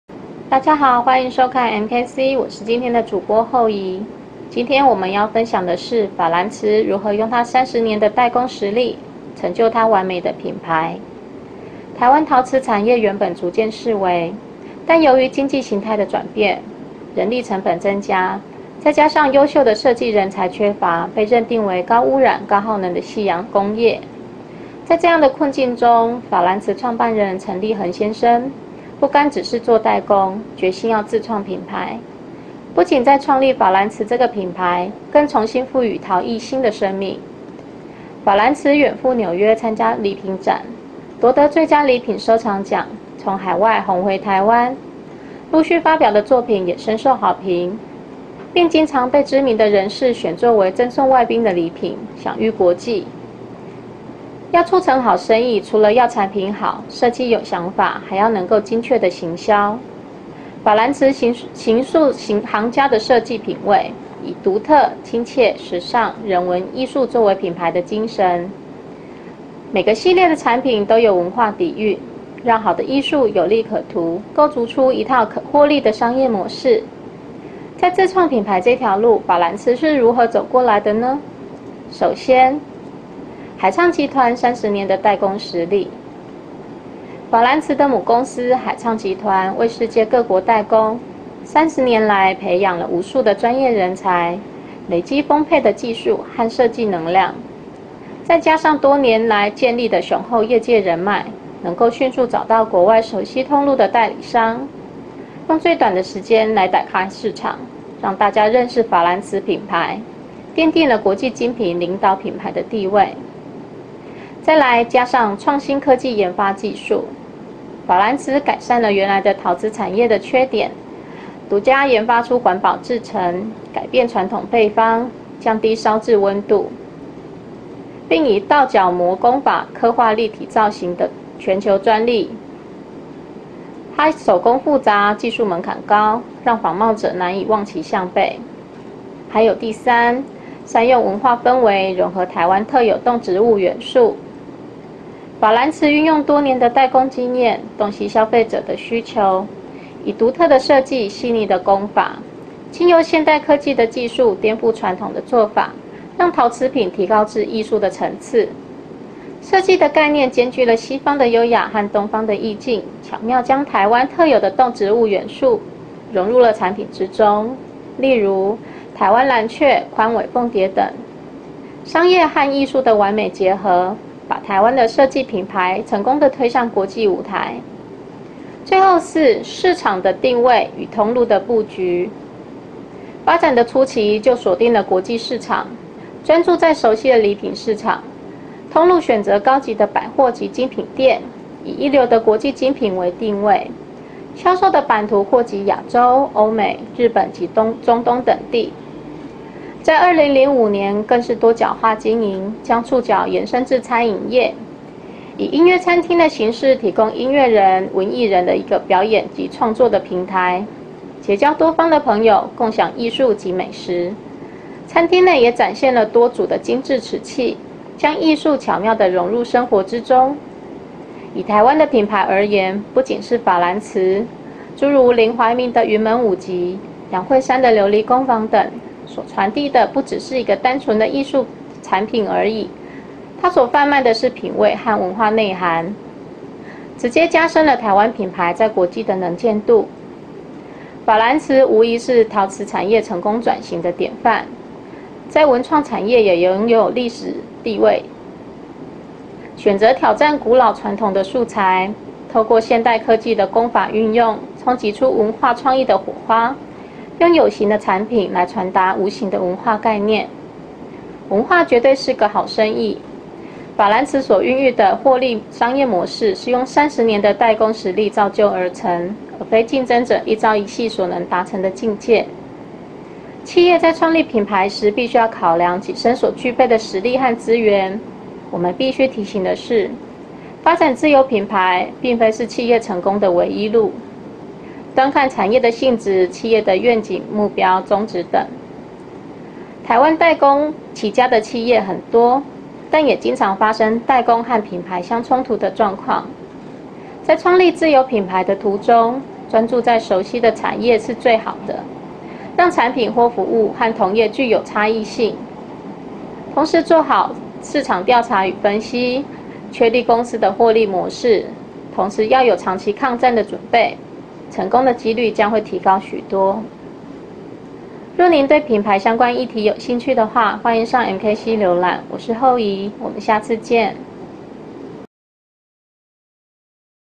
30年代工實力成就法藍瓷完美品牌
2013年7月參與了中小企業領袖研究班之法藍瓷企業參訪行程。海暢集團在2001年時創立法藍瓷品牌並重新賦予陶藝新生命，憑藉三十年的代工經驗與累積的人脈基礎，創新科技研發技術，善用文化氛圍，融合台灣特有動植物元素，將台灣藍鵲、寬尾鳯蝶等加入產品中。現今法藍瓷全球版圖擴及亞洲、歐美、日本及中東等地，擁有6000個以上銷售據點，常獲國內外獎項肯定，客戶禮品首選，更成功推廣台灣設計品牌及文化，實為自創品牌之典範，成為自有品牌的標竿。